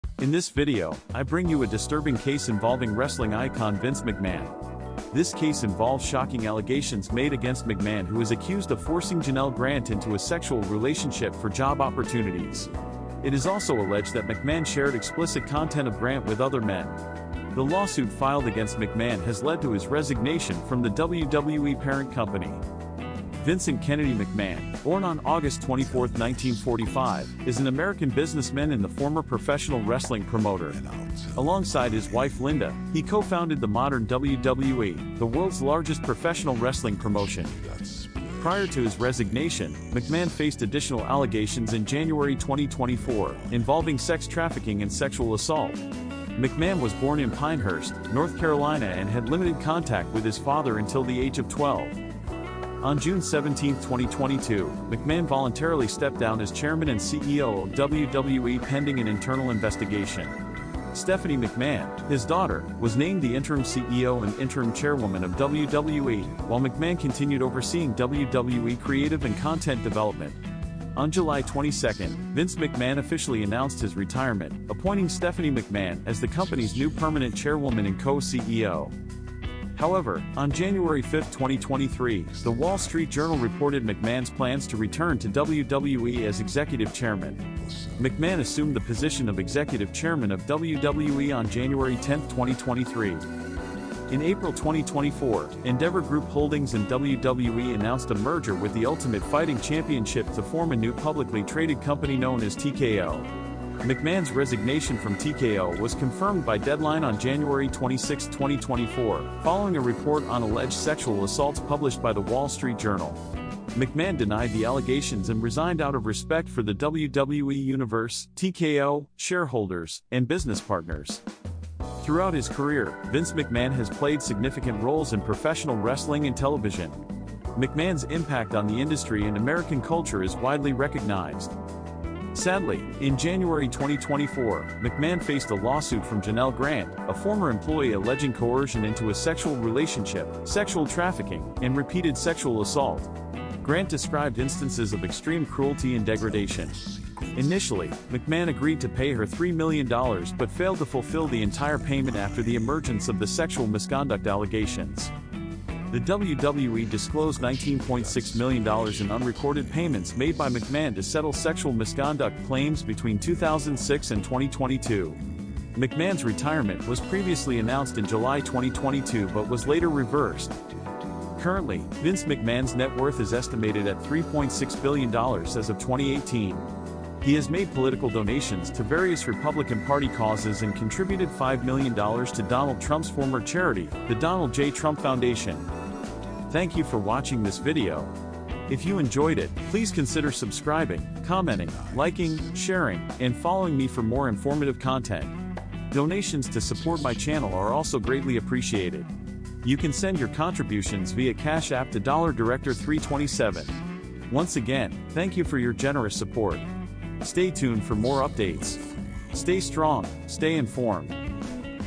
"Unveiling the Disturbing Secrets of Vince McMahon: A Shocking Allegation Exposed"
In this video, I bring you a disturbing case that has recently come to light involving wrestling icon Vince McMahon. This case involves shocking allegations made against McMahon, now 78 years old, accused of forcing Janel Grant, a former employee, into a sexual relationship to get and retain a job. Find out more about this scandal in this video.

It is also alleged that McMahon shared explicit pictures and videos of Grant with other men, including WWE employees. The lawsuit filed against McMahon has led to his resignation from the WWE parent company, marking a major turning point in his career. Learn about Vincent Kennedy McMahon's journey in the world of professional wrestling and media.

Born on August 24, 1945, in Pinehurst, North Carolina, Vincent McMahon is an American businessman, media proprietor, sports executive, and former professional wrestling promoter. Alongside his wife Linda, he co-founded the modern WWE, the world's largest professional wrestling promotion. Discover the background and rise of this influential figure in sports entertainment.

Despite his resignation, McMahon faced additional allegations in January 2024, involving sex trafficking and sexual assault. Get the details on these shocking claims that sent shockwaves through the industry. Learn how Stephanie McMahon, his daughter, stepped in as the interim CEO and chairwoman of WWE while McMahon continued overseeing WWE creative and content development.

Witness the pivotal moment when Vince McMahon officially announced his retirement, appointing Stephanie McMahon as the company's new permanent chairwoman and Co-CEO alongside Nick Khan. Discover the implications of this transition and the possible future of the WWE under new leadership.

However, fate took an unexpected turn with The Wall Street Journal reporting McMahon's plans to return as executive chairman and explore a potential sale of the company. Dive into the events leading up to this announcement and the ensuing media rights negotiations in 2024.

In April 2024, a merger between Endeavor Group Holdings, WWE, and the Ultimate Fighting Championship (UFC) led to the formation of a new publicly traded company known as "TKO." Discover how McMahon's resignation from TKO after allegations of sexual assaults emerged.

Throughout his career, Vince McMahon has left an indelible mark on professional wrestling and television. Explore his role as a TV pioneer, revolutionizing the industry by introducing edgier anti-heroes and changing the landscape of wrestling as we know it.

Sadly, in January 2024, a lawsuit was filed against McMahon by Janel Grant, a former employee who alleged coercion into a sexual relationship, sexual trafficking, and repeated sexual assault. Witness the disturbing details of Grant's allegations, including instances of extreme cruelty and degradation. Learn about McMahon's initial agreement to pay $3 million in return for a non-disclosure agreement (NDA) and the fallout that followed.

Additionally, the WWE disclosed $19.6 million in unrecorded payments made by McMahon to settle sexual misconduct claims between 2006 and 2022, shedding light on the extent of the issue within the organization. Delve into the controversy surrounding these undisclosed settlements and the impact on McMahon's retirement plans.

Currently, Vince McMahon's net worth is estimated at $3.6 billion as of 2018. Explore his political donations to Republican Party causes and his contribution of $5 million to Donald Trump's former charity, the Donald J. Trump Foundation.

Thank you for taking the time to watch this video. Once again, thank you for your generous support. Stay strong, stay informed.